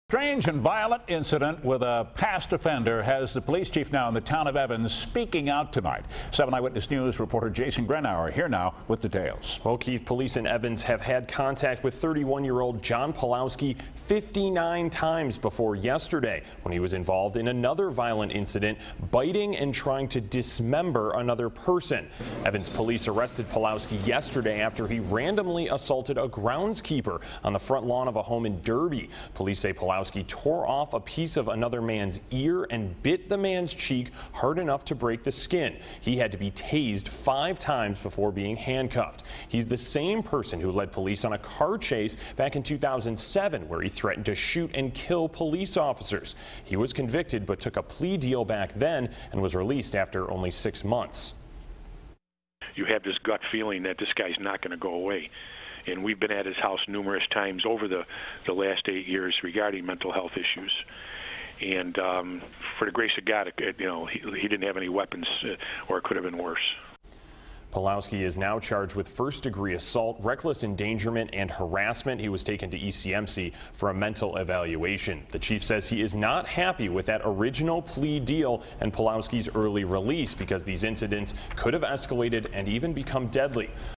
Man bites groundskeeper's face in random attack
A man bit off a part of a groundskeeper’s face and tore his ear before being restrained by officers, police say.

Officers on patrol received a 911 call about a man down in the front yard of a home. Upon arrival, officers say they saw two people attempting to pull John Pawlowski off the groundskeeper.